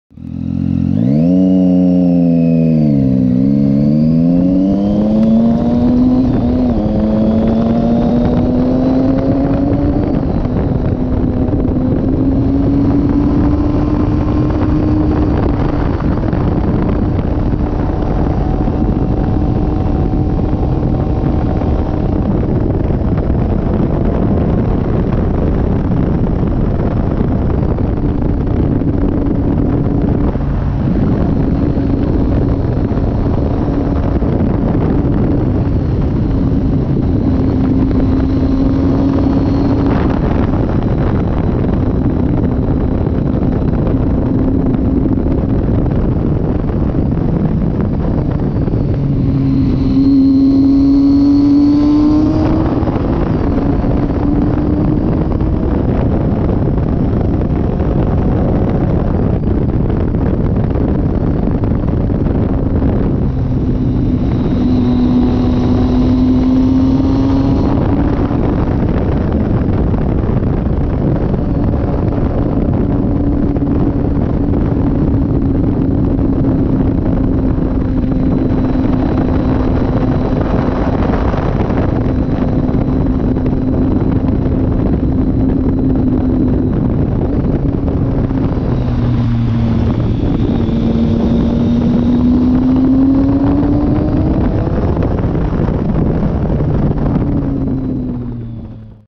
Lovedale Road
Lovedale Road in Elizabeth, PA. There's a few potholes/rough spots, but overall fun twisties. Have to wait on the side of the road to let cars get far enough ahead if I actually want to ride at a decent pace.